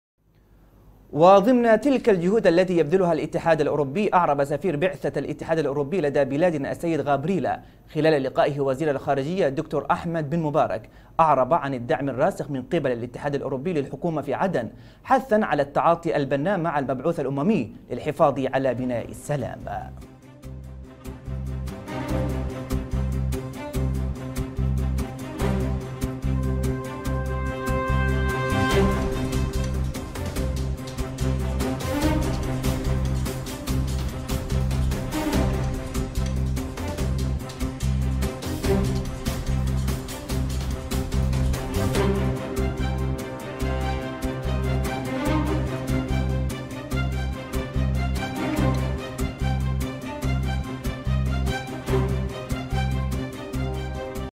خبر | بن مبارك يلتقي سفير بعثة الاتحاد الأوروبي لدى اليمن
ضمن تلك الجهود التي يبذلها الاتحاد الأوروبي أعرب سفير بعثة الاتحاد الأوروبي لدى بلادنا السيد غابرييل خلال لقائه وزير الخارجية الدكتور أحمد بن مبارك أعرب عن الدعم الراسخ من قبل الاتحاد الأوروبي للحكومة في عدن حاثاً على التعاطي البنّاء مع المبعوث الأممي للحفاظ على بِناء السلام

تابعنا على شبكات التواصل الإجتماعي:
ــــــــــــــــــــــــــــــــــــــــــــــــــــــــــ